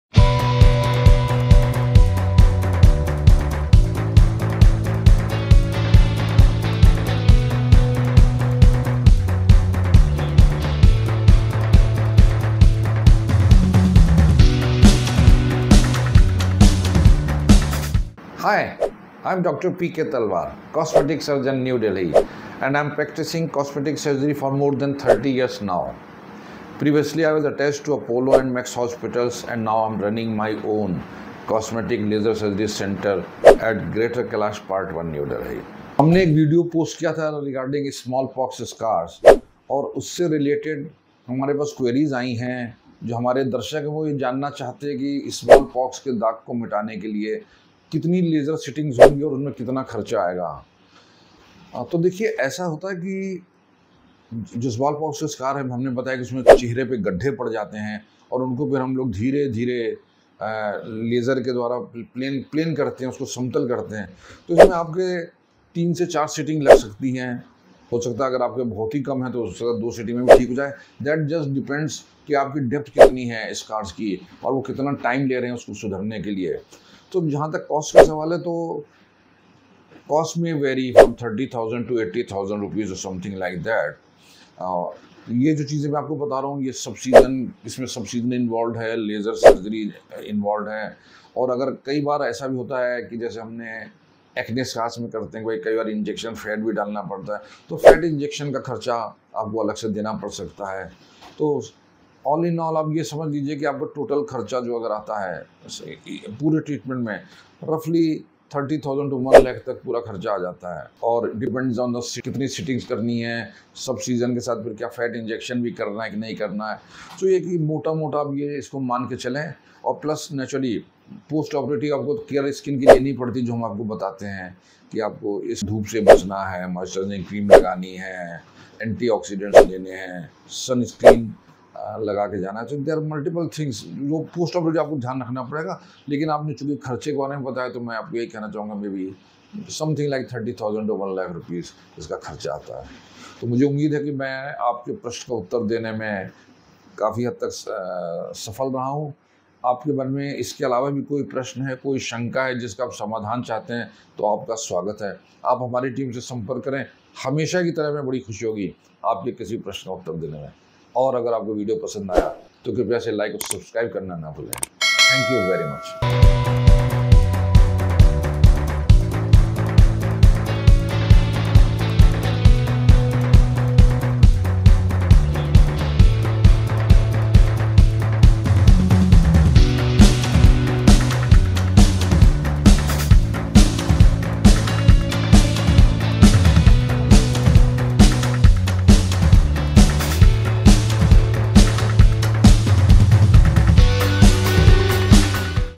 SmallPox Scar Removal Cost | Scar Removal Treatment In Delhi | Dr. PK Talwar
In India, the cost of removing smallpox scars varies depending on the treatment method chosen. Options include topical treatments, laser therapies, dermal fillers, microneedling, chemical peels, and surgical procedures. Each method has its own approach and effectiveness, with choices ranging from non-invasive to more intensive procedures. Consulting with a qualified dermatologist or cosmetic surgeon can help determine the most suitable treatment for your specific needs and provide a tailored recommendation.

Address: E-34, Greater Kailash – I,